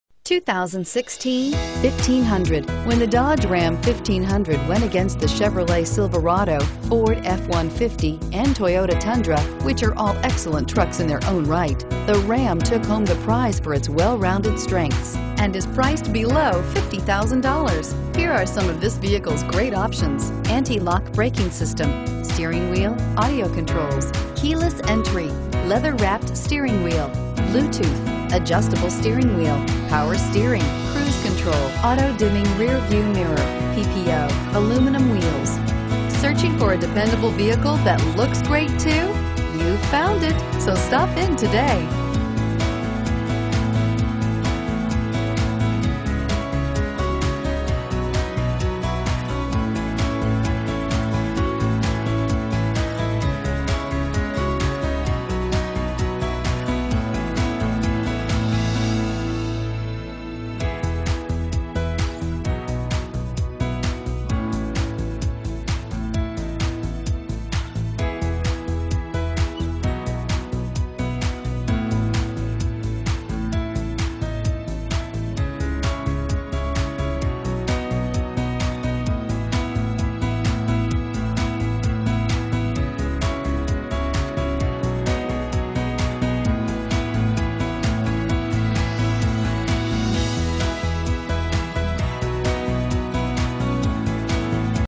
2016 Ram 1500 27225
2016 RAM 1500 Roswell, GA
Stock# 27225

Palmer Dodge
11460 Alpharetta Highways

4WD. Short Bed! Crew Cab!

This outstanding 2016 Dodge Ram 1500 is the dealer maintained truck you've been looking for. It has plenty of passenger space and cargo room galore.